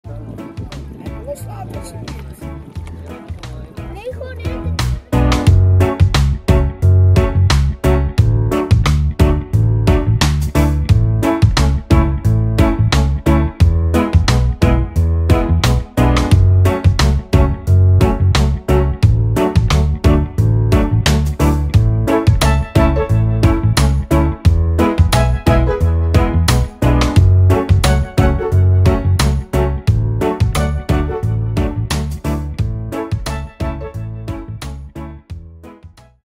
Kids from Breda and Yangzhou play on water playgrounds
The water playground given as a gift by Breda, Yangzhou's sister city in the Netherlands, opened to the public on Dec 21 in Canal Sanwan Scenic Area. The same water playground was also built in Breda. Look at the kids playing happily on the playgrounds!